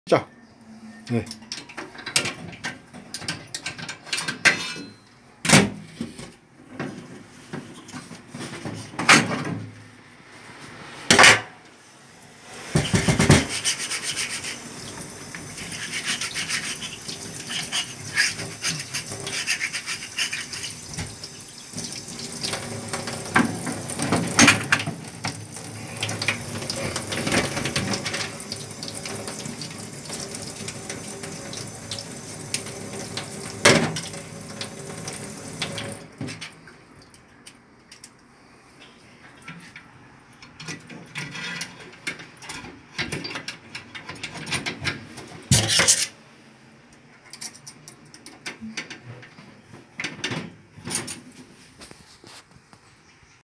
김밥기계 절단기 청소장면
김밥기계 절단기를 손쉽게 청소를 할수 있습니다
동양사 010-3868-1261